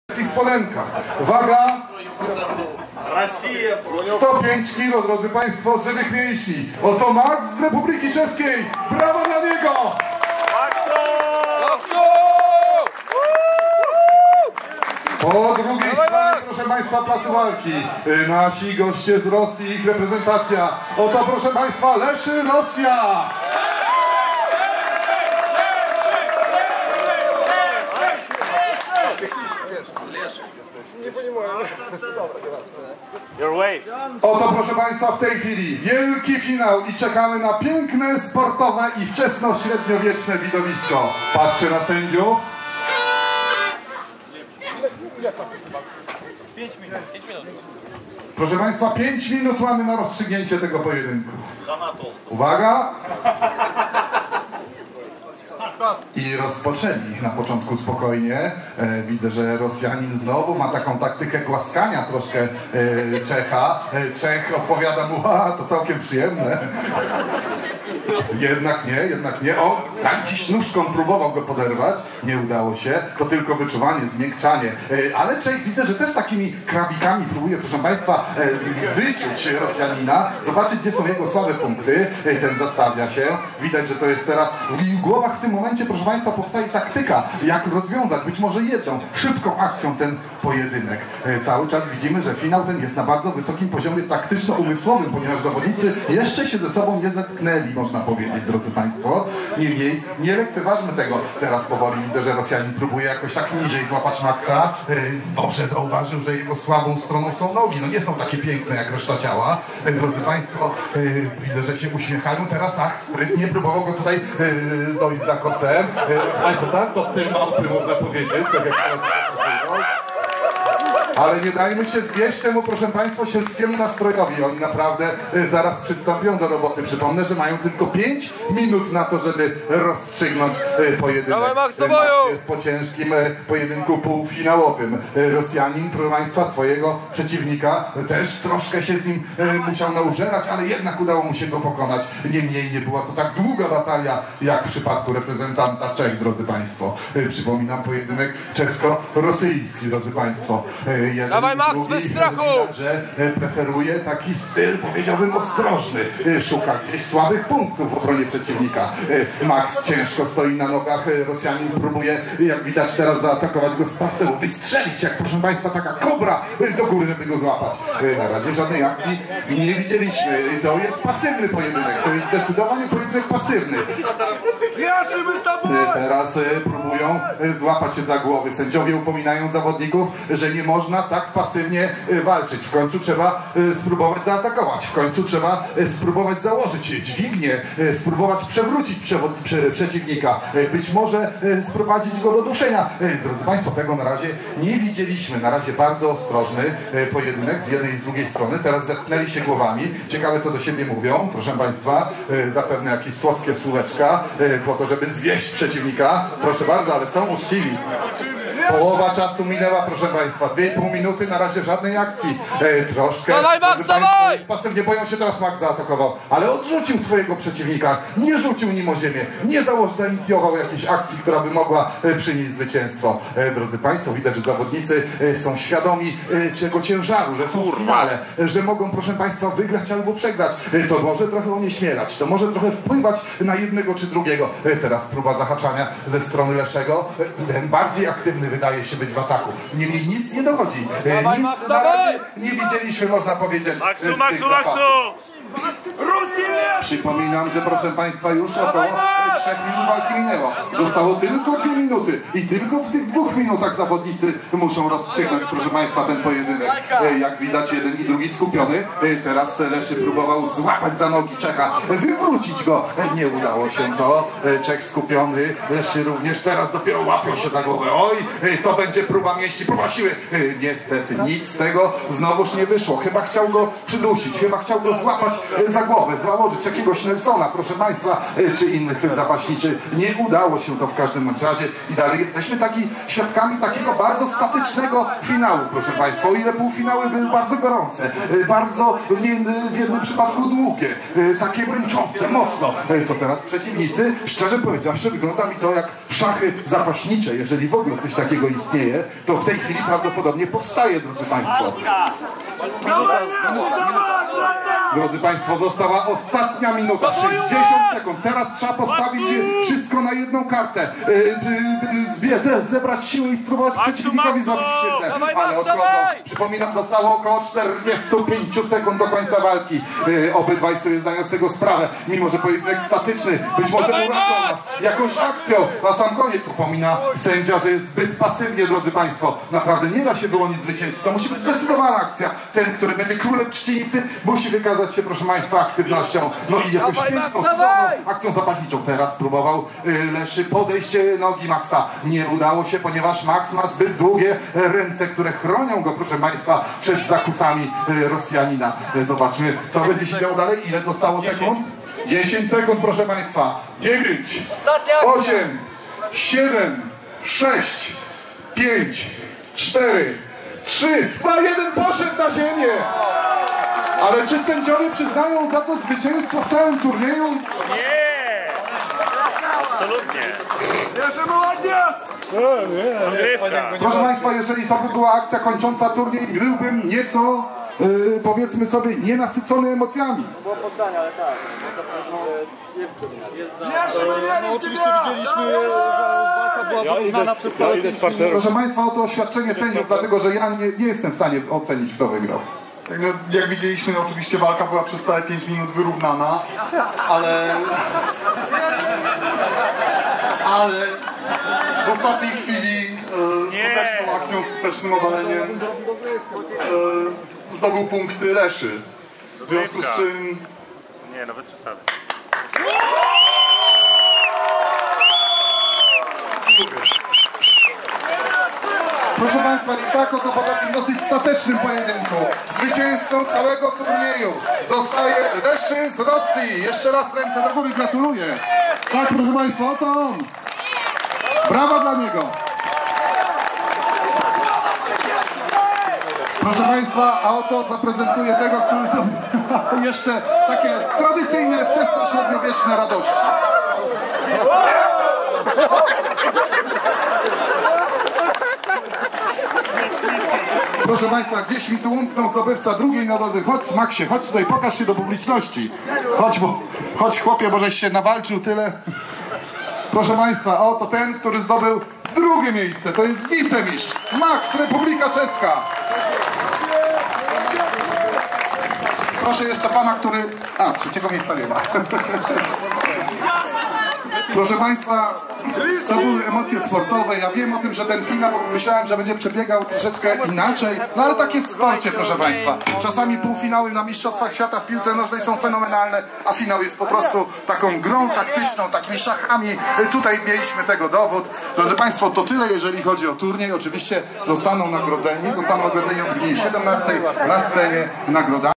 Karpatska Troja w Trzcinicy 2015. Zapasy.
Turnej o nagrodę specjalną.
Фестиваль в Тщчинице. Карпатская Троя 2015. Финал троеборья за специальную награду. Выиграл Владислав "Леший" Бунеску, г.Гагарин.